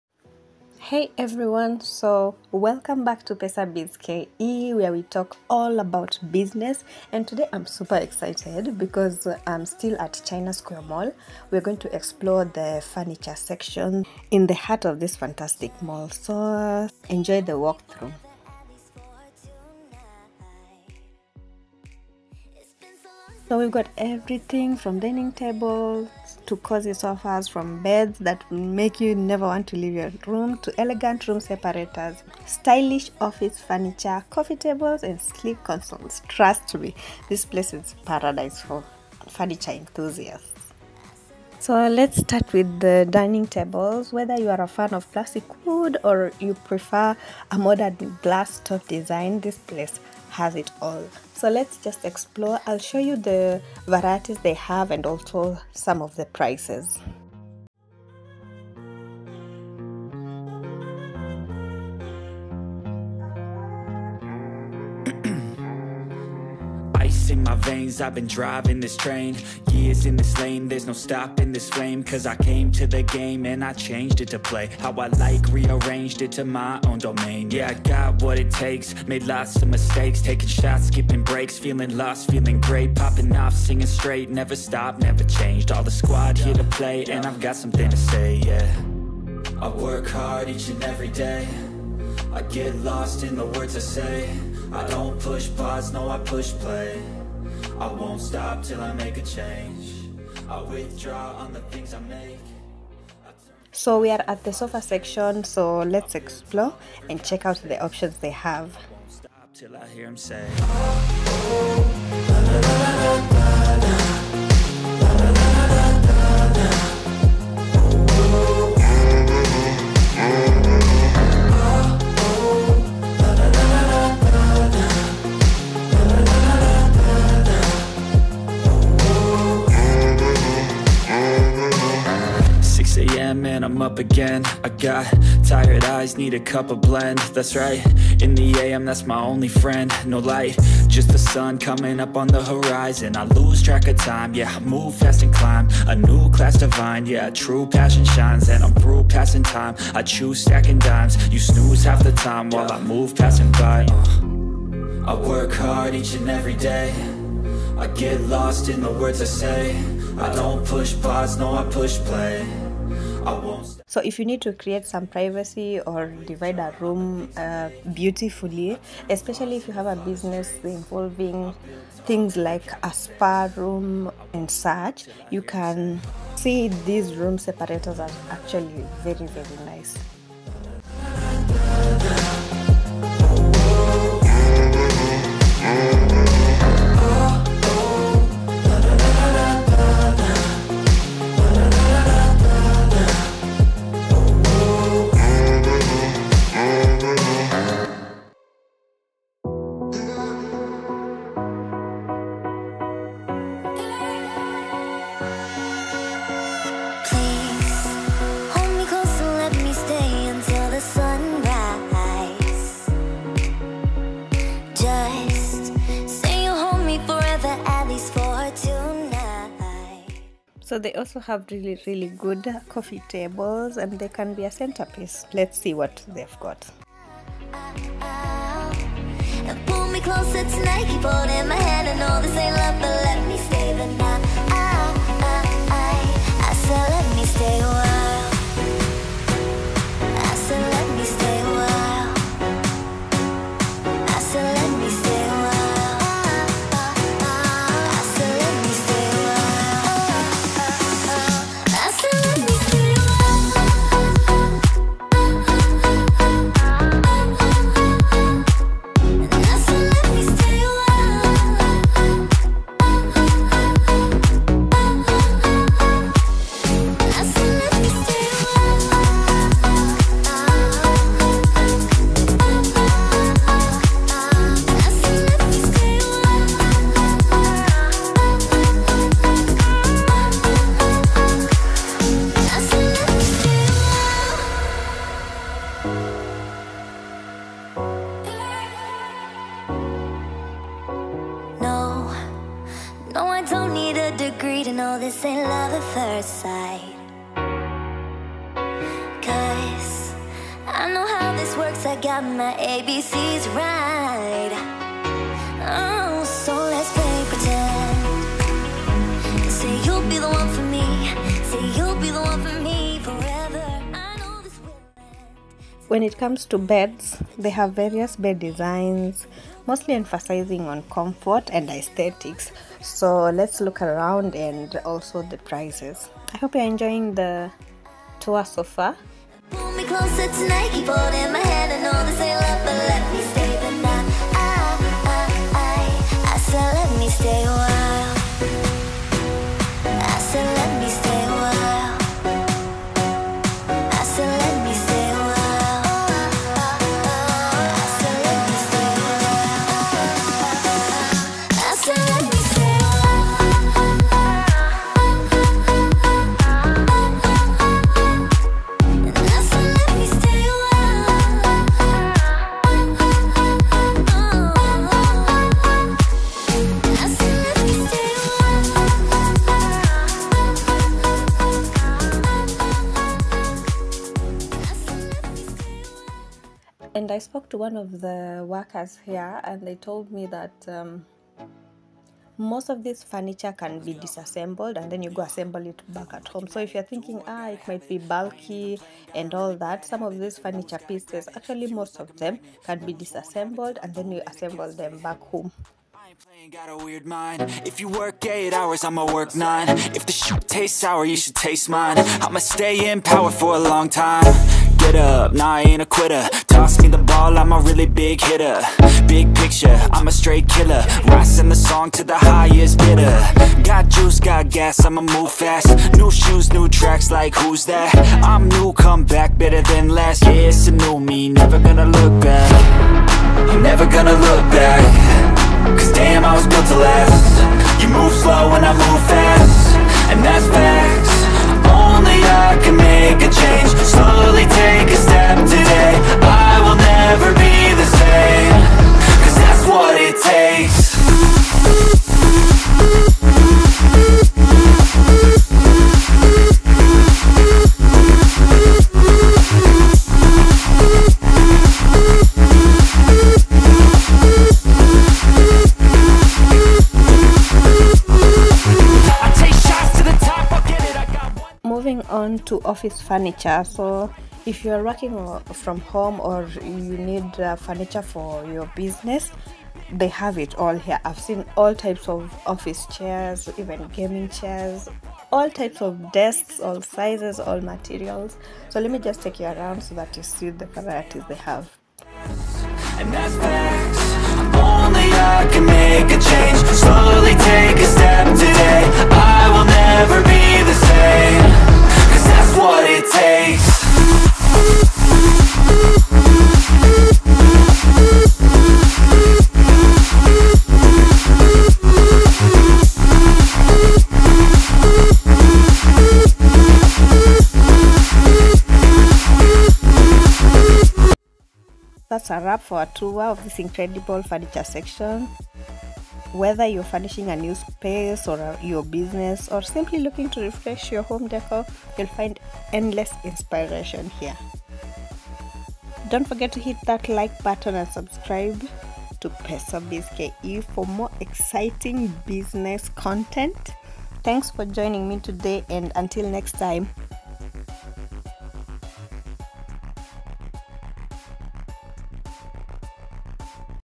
VERY AFFORDABLE FURNITURE AT CHINA SQUARE MALL!
Welcome to China Square's Furniture Section – your one-stop destination for all things home décor and furnishings! Join us on an exciting tour as we explore the sprawling aisles of this mall's furniture wonderland. From contemporary designs to timeless classics, we'll uncover hidden gems and budget-friendly finds.

Salome M,
Founder, Pesabiz Ke